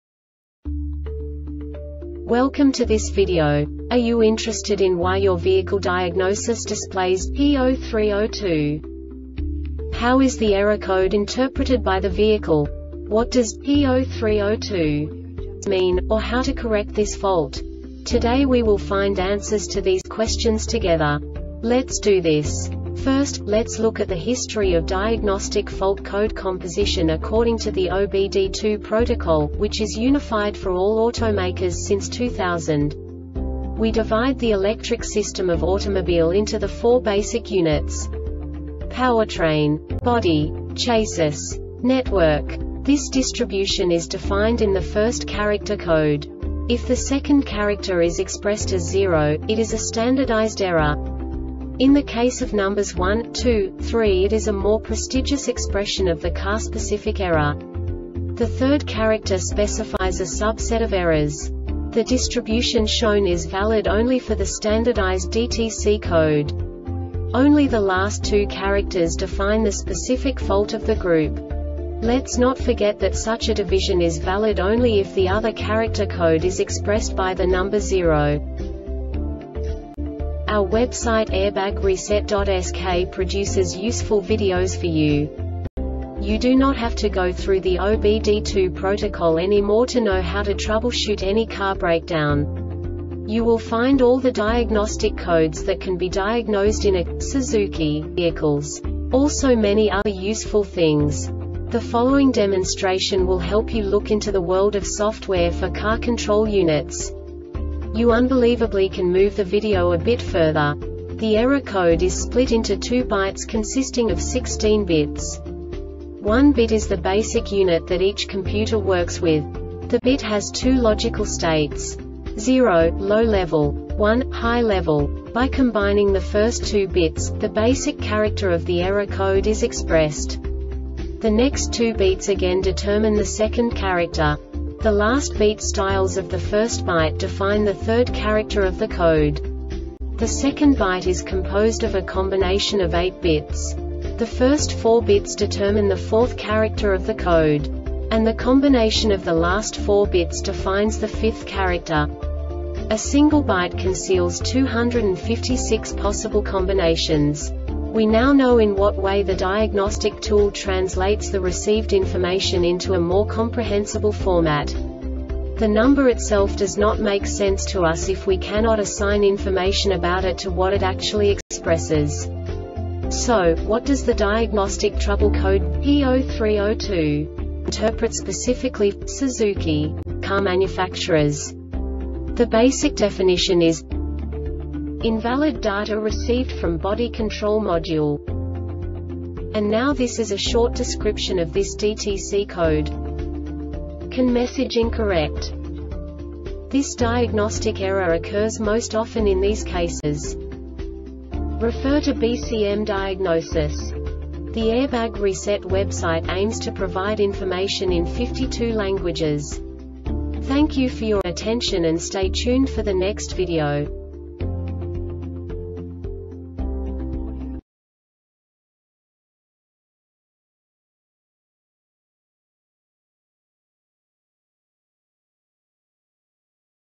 DTC Suzuki P0302 Short Explanation
Contents:
0:21 Basic DTC analysis according to OBD2 protocol standard.
1:48 Insight into programming
2:58 A diagnostically understandable description of the Diagnostic Trouble Code
3:05 Basic error definition

Make:
Suzuki
Code:
P0302
Definition:
Invalid Data Received From Body Control Module
Description:
CAN message incorrect.
Cause:
 Refer to BCM diagnosis